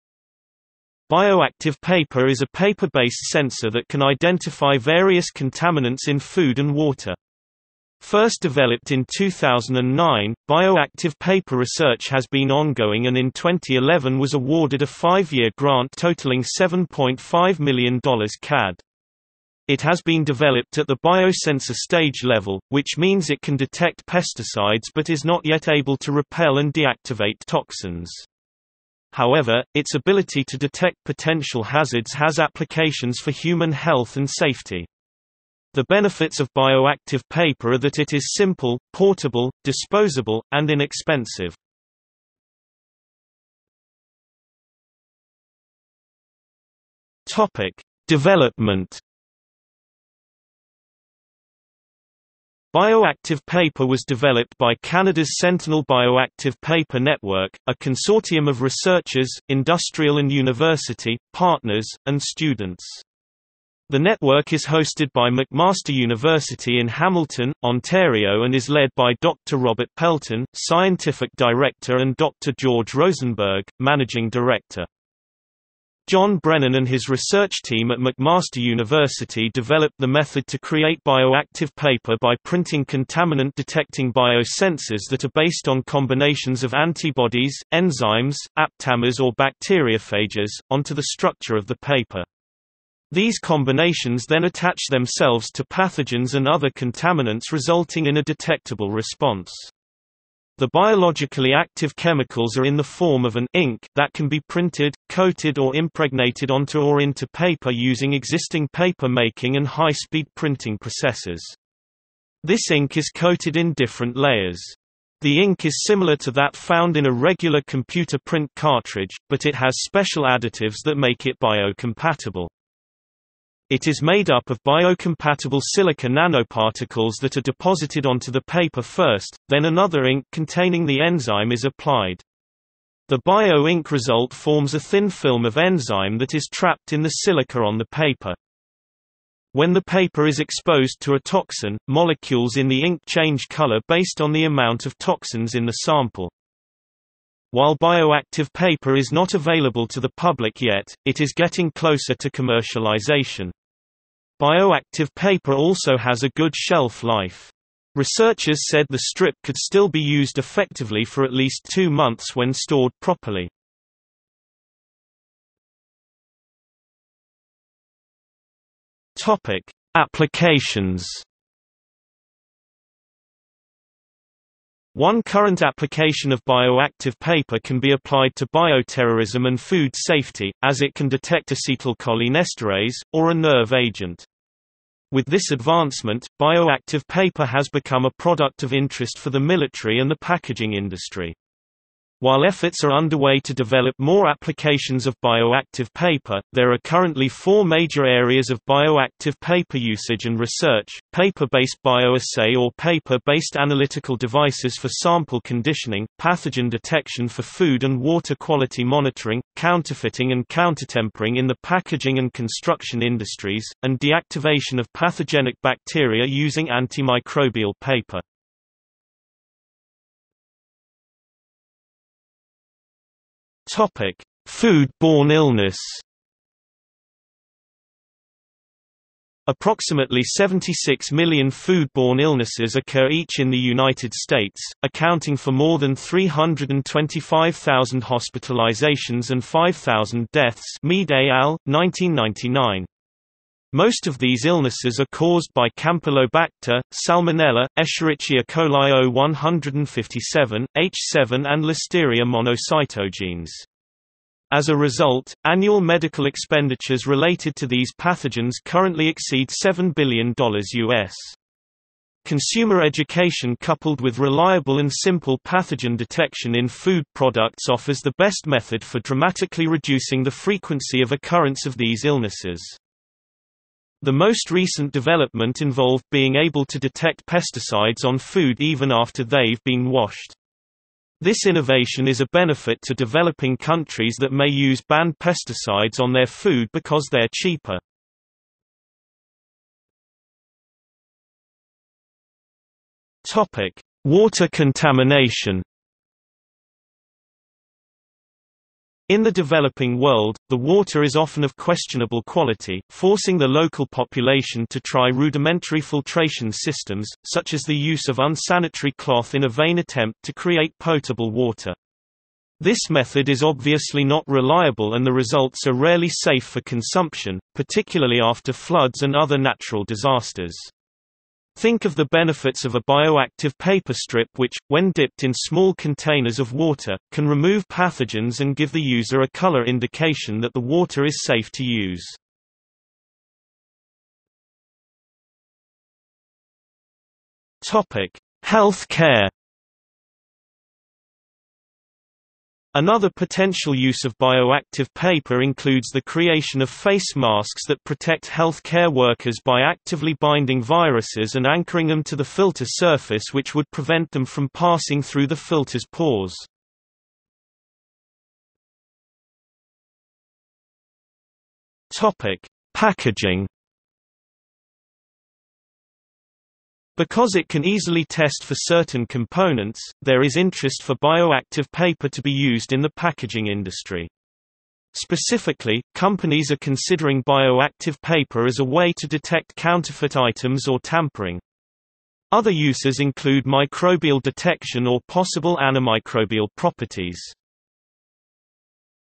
Bioactive paper | Wikipedia audio article
00:00:43 1 Development
00:02:45 2 Applications
00:03:40 2.1 Food-borne illness
00:04:49 2.2 Water contamination
00:05:34 2.3 Health care
00:05:58 2.4 Packaging

- Socrates

SUMMARY
Bioactive paper is a paper-based sensor that can identify various contaminants in food and water. First developed in 2009, bioactive paper research has been ongoing and in 2011 was awarded a 5-year grant totalling $7.5 million CAD. It has been developed at the biosensor stage level, which means it can detect pesticides but is not yet able to repel and deactivate toxins. However, its ability to detect potential hazards has applications for human health and safety. The benefits of bioactive paper are that it is simple, portable, disposable, and inexpensive.